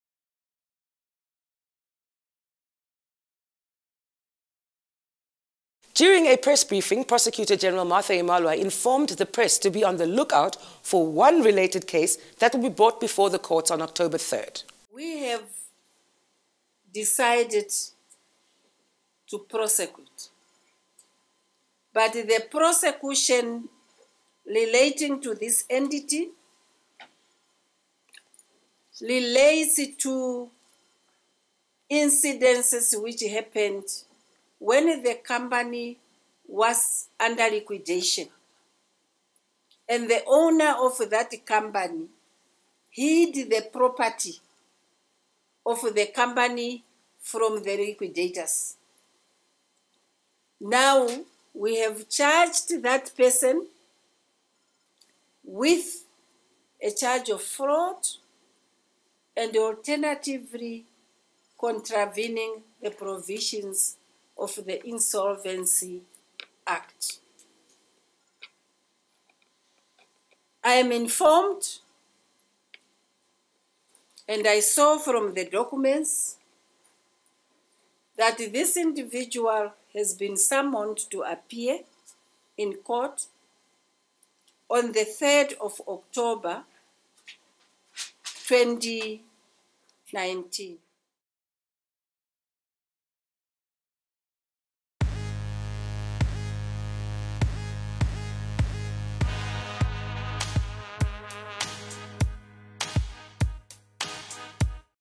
It's A Wrap: GIPF Scandal
During the press briefing, Prosecutor General Martha Imalwa informed the press to be on the look out for the one related case that will be brought before the courts on October 3.